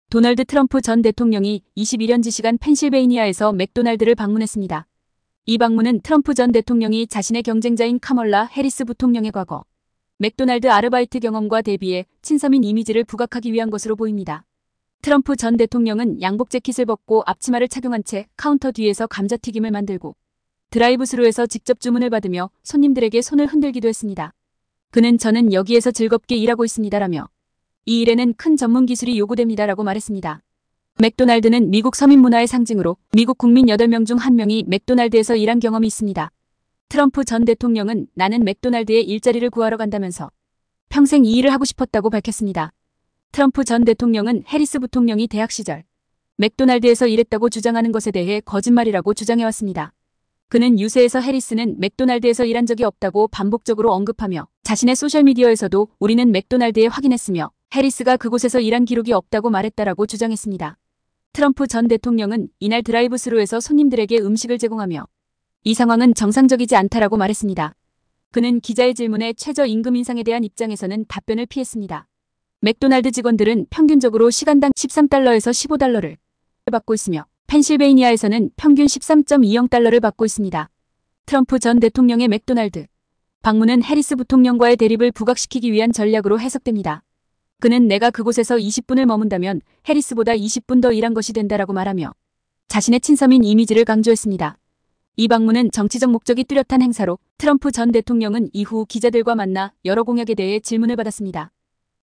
트럼프가 튀긴 감자튀김, 해리스에게 튀는 반격의 신호탄
미국 공화당 대선 후보인 도널드 트럼프 전 대통령이 20일(현지시간) 대선 핵심 경합주인 펜실베이니아에서 햄버거 체인인 맥도날드를 방문했습니다. AP통신과 뉴욕타임스(NYT) 등은 트럼프 전 대통령이 이날 오후 펜실베이니아주 필라델피아 북부의 벅스 카운티 피스터빌-트레버스에 있는 한 맥도날드 매장을 찾았다고 보도했습니다.
트럼프 전 대통령은 양복 재킷을 벗고 앞치마를 착용한 채 카운터 뒤에서 감자튀김을 만들었습니다. 또한 드라이브스루에서 직접 주문을 받으며 창문으로 길 건너에 서 있는 사람들에게 손을 흔들기도 했습니다. 드라이브스루에서 기다리던 한 손님은 “트럼프 2024”라고 외치며 호응했습니다.

트럼프 전 대통령은 언론에 “저는 여기에서 즐겁게 (일하고) 있습니다”라며 “이 일에는 큰 전문기술이 요구됩니다”라고 말했습니다. 맥도날드는 미국 서민 문화의 상징으로, 미국 국민 8명 중 1명이 맥도날드에서 일한 경험이 있습니다. 트럼프 전 대통령은 같은 이유로 맥도날드 매장을 방문하고 싶다고 요청했다고 알려졌습니다.
트럼프 전 대통령은 펜실베이니아에 도착하자마자 언론에 “나는 맥도날드에 일자리를 구하러 간다”면서 “나는 평생 이 일을 하고 싶었다”고 밝혔습니다. 그의 맥도날드 방문은 해리스 부통령과의 대립각을 세우기 위한 의도로 해석됩니다.

해리스 부통령은 지난 7월 대학 시절 맥도날드에서 아르바이트한 경험을 강조하며 친서민 이미지를 부각해왔습니다. 그러나 트럼프 전 대통령은 유세에서 “해리스는 맥도날드에서 일했다고 거짓말을 했다”고 주장해왔지만, 구체적인 근거는 제시하지 않았습니다. 트럼프 전 대통령은 이날 자신의 소셜미디어(SNS)에서 “우리는 맥도날드에 확인했으며, 해리스가 그곳에서 일한 기록이 없다고 말했다”라고 주장했습니다.
트럼프 전 대통령은 이날 맥도날드 매장에서 감자튀김을 튀기며 드라이브스루에서 주문을 받았습니다. 그는 “저는 여기에서 즐겁게 일하고 있습니다”라고 말하며, 자신이 진정한 친서민 후보임을 강조하기 위해 맥도날드를 방문한 것으로 보입니다. 그는 “나는 맥도날드에 일자리를 구하러 갔다”라고 덧붙였습니다.

트럼프 전 대통령은 해리스 부통령의 맥도날드 아르바이트 발언에 대해 반복적으로 거짓말이라고 주장해왔습니다. 해리스 부통령은 대학 시절 맥도날드에서 일한 경험을 통해 중산층 출신임을 강조하고 있습니다. 트럼프 전 대통령은 “내가 그곳에서 20분을 머문다면 내가 해리스보다 20분 더 그곳에서 일한 것이 된다”라고 말했습니다.

도널드 트럼프 전 대통령이 맥도날드를 방문하여 감자튀김을 튀기고 드라이브스루에서 손님들에게 음식을 제공했습니다. 그는 “저는 여기에서 즐겁게 일하고 있습니다”라고 밝혔습니다. 트럼프 전 대통령은 이 방문에서 해리스 부통령이 맥도날드에서 일한 적이 없다고 주장하며, 그의 주장을 뒷받침할 근거는 제시하지 않았습니다
그는 드라이브스루 창문에서 지지자들에게 손을 흔들며 “이 상황은 정상적이지 않다”라고 말했습니다. 트럼프 전 대통령은 맥도날드 직원들이 열심히 일하고 있다고 칭찬하며, 최저 임금 인상에 대한 질문에는 답변을 피했습니다. 트럼프 전 대통령은 이후 펜실베이니아에서 열린 타운홀 미팅에서 이민과 세금 면제 등 주요 공약에 대해 지지자들과 질문을 주고받았습니다.